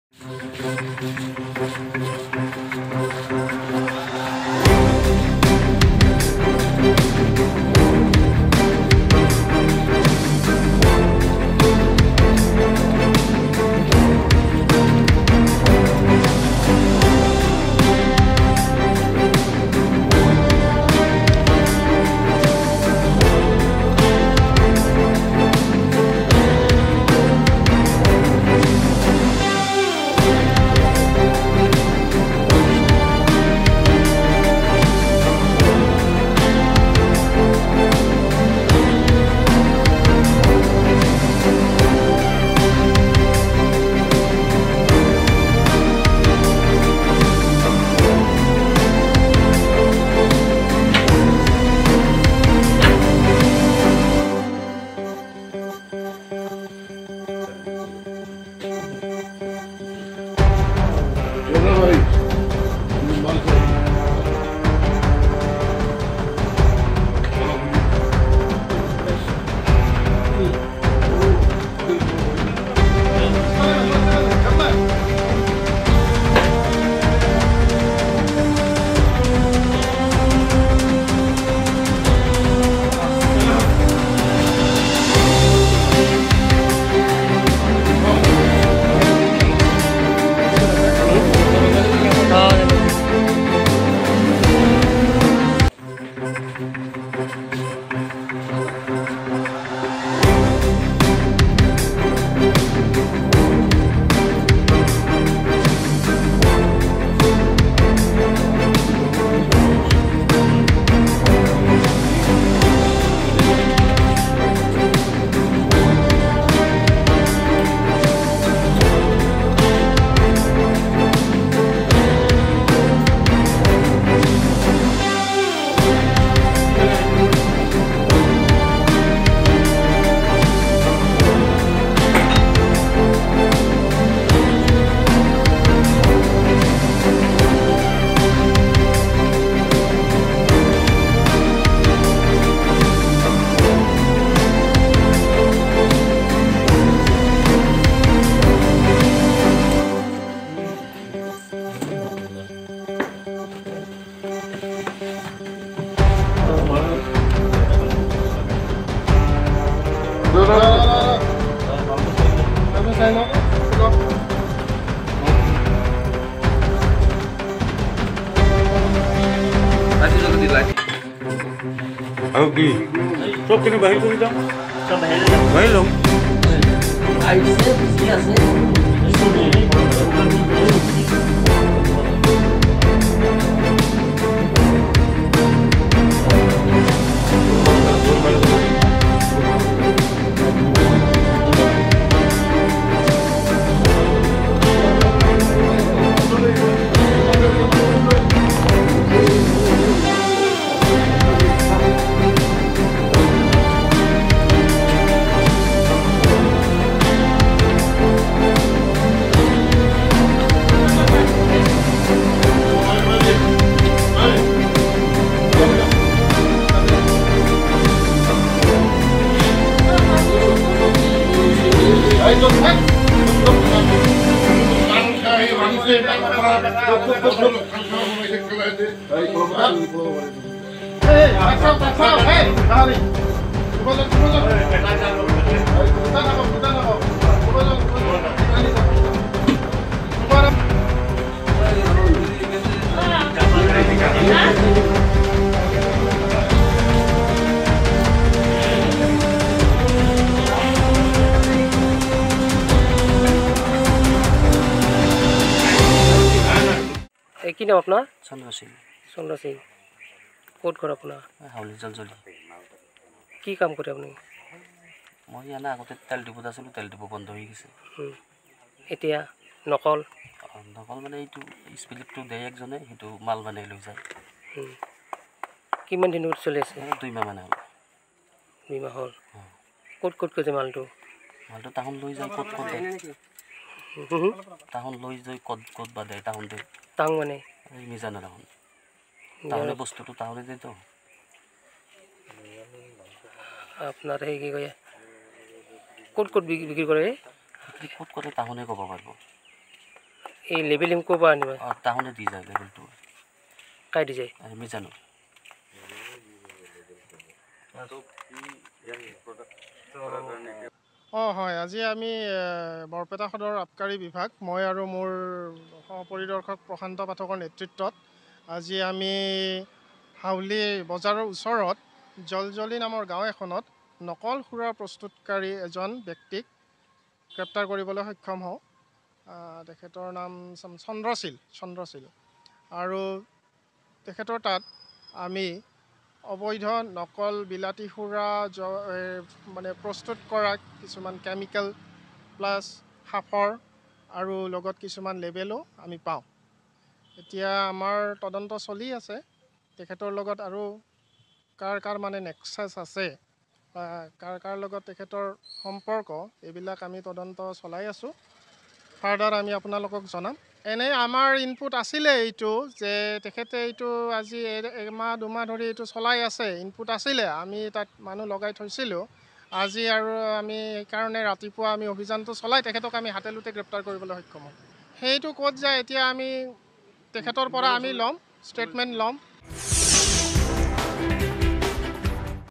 বৰপেটাৰ হাউলীত ভেজাল সুৰা প্ৰস্তুত কৰা কাৰখানা উৎখাত ৷চন্দ্ৰ শীল নামৰ এজনক গ্ৰেপ্তাৰ আৱকাৰীৰ ।
বৰপেটাৰ হাউলীত ভেজাল সুৰা প্ৰস্তুত কৰা কাৰখানা উৎখাত ৷চন্দ্ৰ শীল নামৰ এজনক গ্ৰেপ্তাৰ  আৱকাৰীৰ ।বৃহৎ আকাৰৰ ড্ৰামত বৰষুণৰ পানী জমা কৰি  স্পিৰিট মিহলি কৰি প্ৰস্তুত কৰা  হৈছিল নকল আৰু ভেজাল সুৰা ৷